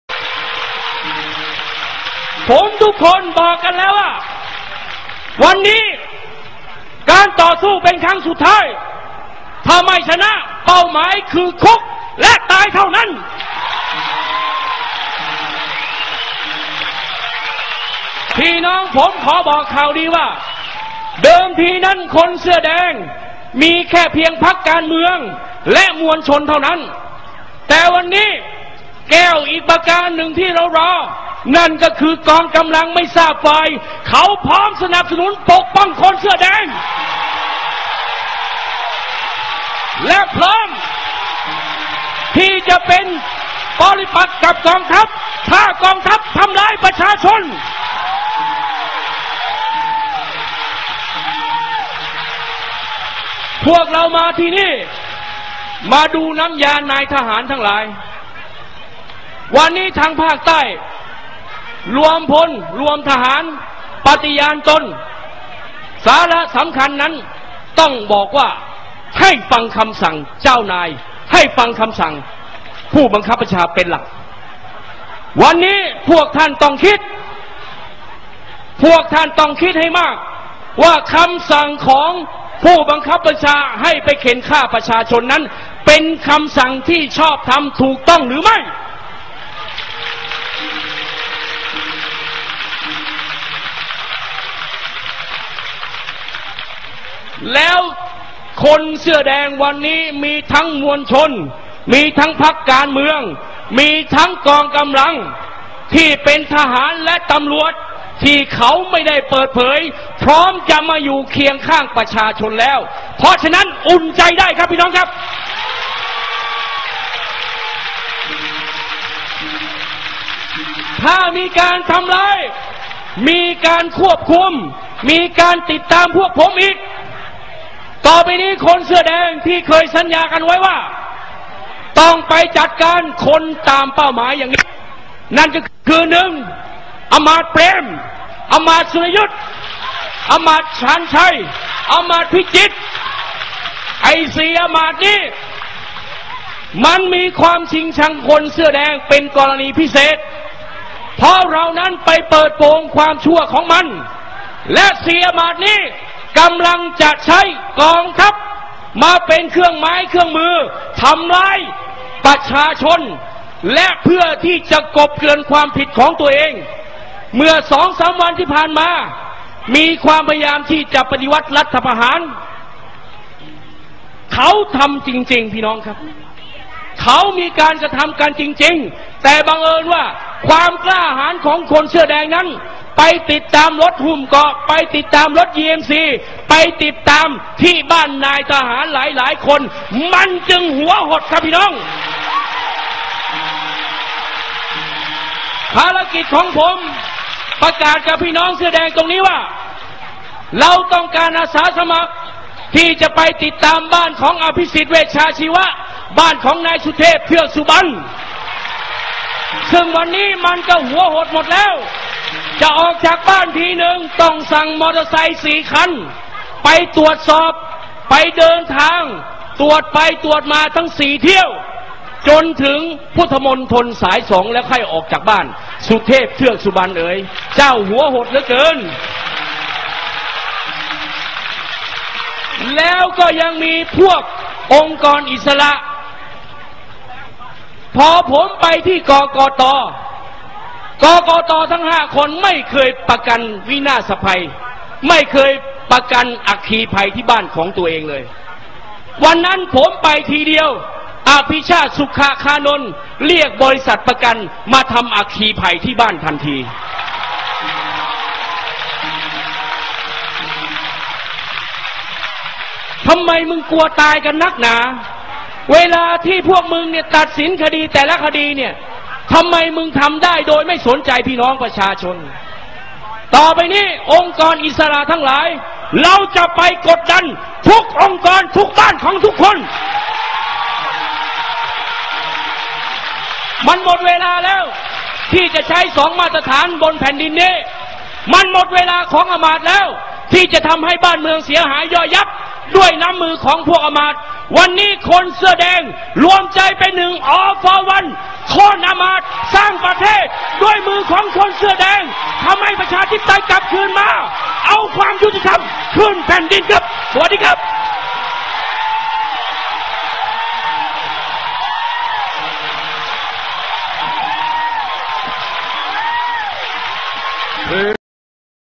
กี้ร์ ปูดมีกองกำลังช่วยเสื้อแดง
ติดตามเรื่องราวในหมวด".. เมษาเลือด 10 เม.ย.2553" ได้ที่

อริสมันต์ ปูดมีกองกำลังลับแอบช่วยเหลือสนับสนุนการชุมนุมของกลุ่ม นปช. หรือคนเสื้อแดง คลิปจากข่าวตามไปหาอ่านในบล๊อคชบา

โพสเมื่อวันที่ 15 เมษายน 2553 หลังคนเสื้อแดงก่อเหตุก่อการณ์ร้าย ป่วนเมืองเมื่อวันที่ 10 เมษายน 2553
โดยมีกองกำลังไม่ทราบฝ่าย ที่เดินปะปนอยู่ในกลุ่มคนเสื้อแดง และใส่เสื้อผ้าที่ทำให้คิดได้ว่า เป็นการ์ดเสื้อแดง ใช้อาวุธสงคราม ยิงทหาร ที่เข้าสลายการชุมนุม ตายไป 4 นาย และพวกคนเสื้อแดงกันเองตายไป 18 ศพ หลังจากที่แพทย์พิสูจน์ว่า พลเรือนทุกราย ถูกยิงจากด้านหลัง มีเพียงรายเดียวที่ถูกยิงจากด้านหน้า (ศรีษะ)ตายคาที่ มีคลิปแฉจาก ศอฉ.